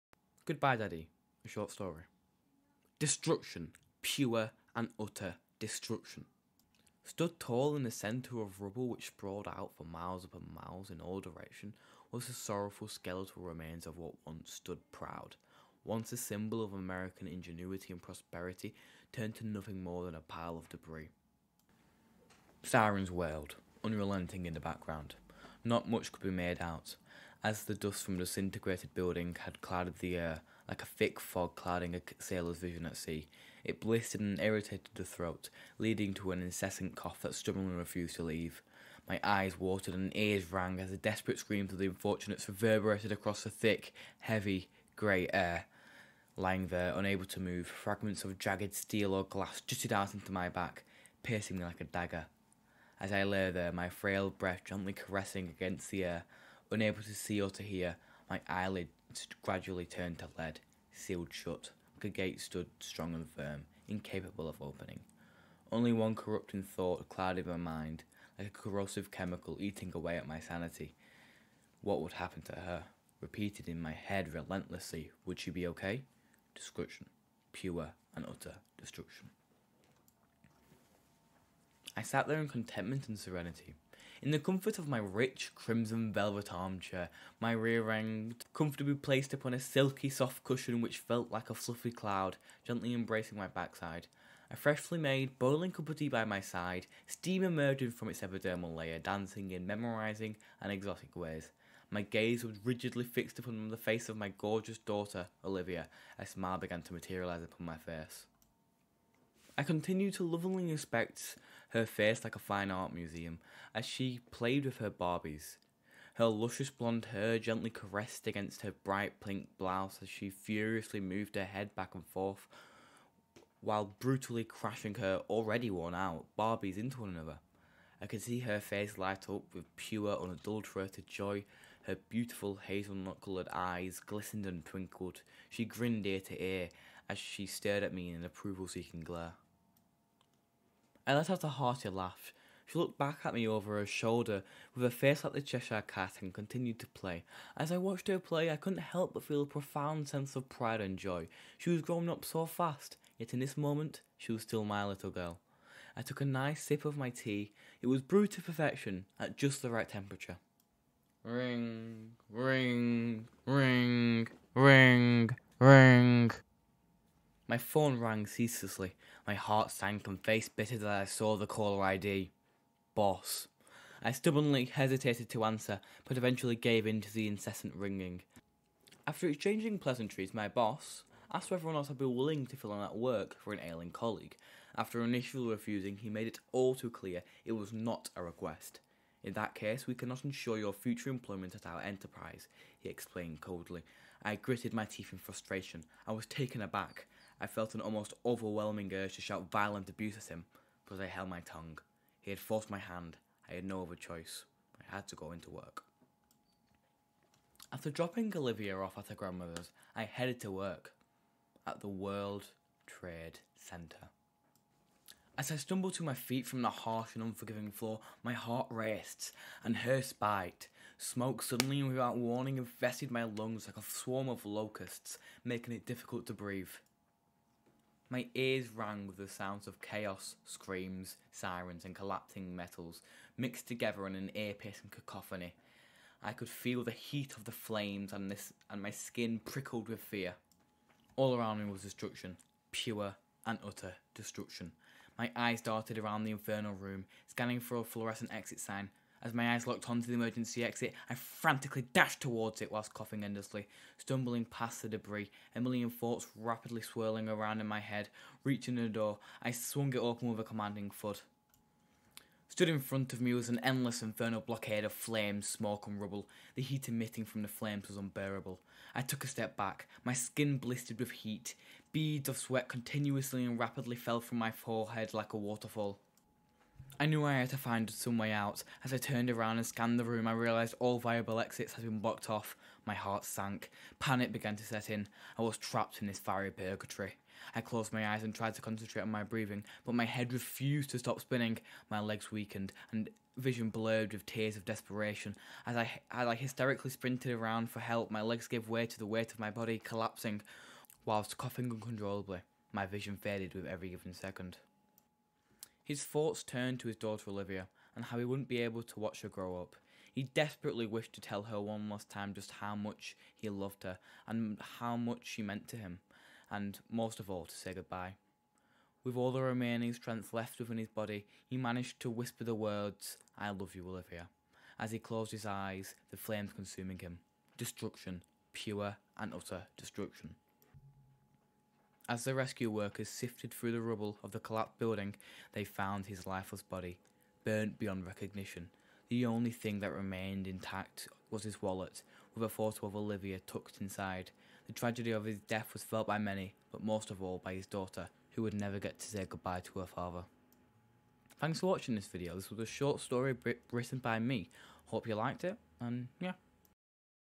Goodbye Daddy....| A Short story
Destruction. Pure and utter destruction.

Stood tall in the centre of a pool of rubble, which sprawled out for miles upon miles in all directions, was the sorrowful skeletal remains of what once stood proud--once a symbol of American ingenuity and prosperity turned to nothing more than a pile of debris. Sirens wailed, unrelenting in the background. Not much could be made out, as dust from the disintegrated building clouded the air, like a thick fog clouding a Sailor’s vision at sea, it blistered and irritated the throat leading to an incessant cough, that stubbornly refused to leave. My eyes watered, and ears rang, as the desperate screams of the unfortunates reverberated across the thick, heavy, grey air. Lying there unable to move, fragments of jagged steel or glass jutted out into my back, piercing me like a dagger. As I lay there, my frail breath gently caressing the air, unable to see or to hear, my eyelids gradually turned to lead; sealed shut, like a gate stood strong and firm- incapable of opening. Only one corrupting thought clouded my mind- like a corrosive chemical eating away at my sanity: “What would happen to her???” Repeated in my head, relentlessly “Would she be okay???” ….
I sat there in contentment and serenity, in the comfort of my rich crimson, velvet armchair, my rear end comfortably placed upon a silky soft cushion- which felt like a fluffy cloud gently embracing my backside. A freshly made, boiling cup of tea by my side steam emerging from its’ epidermal layer; dancing in memorising and exotic ways. My gaze was rigidly fixed upon the face of my gorgeous daughter, Olivia; a smile began to materialise upon my face. I continued to lovingly inspect her face like a fine-art museum, as she played with her barbies. Her luscious blonde hair gently caressed against her bright pink blouse, as she furiously moved her head back and forth while brutally crashing her (already worn-out) barbies into one and other. I could see her face light up with pure, unadulterated joy. Her beautiful, hazelnut-coloured eyes glistened and twinkled; she grinned ear to ear, as she stared at me in an approval-seeking glare. I let out a hearty laugh, she looked back at me over her, shoulder with a face like the Cheshire cat and continued to play. As I watched her play, I couldn't help but feel a profound sense of pride and joy. She was growing up so fast, yet in this moment she was still my little girl. I took a nice sip of my tea, it was at brewed to perfection, and at just the right temperature.
Riiiiiingg. RIIIIING. RIIIIIIIINGG.
My phone rang ceaselessly. My heart sank, and face bittered as I saw the caller ID- “Boss.” I stubbornly hesitated to answer but eventually gave in to the incessant ringing. After exchanging pleasantries, my boss asked whether I would be willing to fill in at work for an ailing colleague. After initially refusing, he made it all too clear it wasn’t a request. “In that case we cannot ensure your future employment at our enterprise” he explained coldly. I gritted my teeth in frustration, I was taken aback. I felt an almost overwhelming urge to shout violent abuse at him, but I held my tongue. He had forced my hand; I had no other choice- I had to go to work.
After dropping Olivia off at her grandmother’s, I headed to work. At the World Trade Centre.
As I stumbled to my feet from the harsh and unforgiving floor, my heart raced, and hairs spiked. Smoke suddenly, and without warning infested my lungs like a swarm of locusts, making it difficult to breathe. My ears rang with the sound of chaos; screams, sirens, and collapsing metal mixed together in an ear-piercing cacophony. I could feel the heat of the flames, and my skin prickled with fear. All around me was destruction- pure and utter destruction. My eyes darted around the infernal room, scanning for a florescent “Exit” sign. As my eyes locked onto the emergency exit, I frantically dashed towards it whilst coughing endlessly, stumbling past the debris. A million thoughts rapidly swirled around my head. Reaching the door, I swung it open with a commanding thud. Stood in front of me was an endless infernal blockade of flames, smoke, and rubble. The heat emitting from the flames was unbearable. I took a step back, my skin blistered with the heat. Beads of sweat continuously and rapidly fell from my forehead- like a waterfall. I knew I had to find some way out, as I turned around and scanned the room, I realised that all viable exits had been blocked off. My heart sank. Panic began to set in- I was trapped in this fiery purgatory. I closed my eyes and tried to concentrate on my breathing, but my head refused to stop spinning. My legs weekend, and vision blurred, with tears of desperation. As I hysterically sprinted around for help, my legs gave way to the weight of my body - collapsing. Whilst coughing uncontrollably. My vision faded with every given.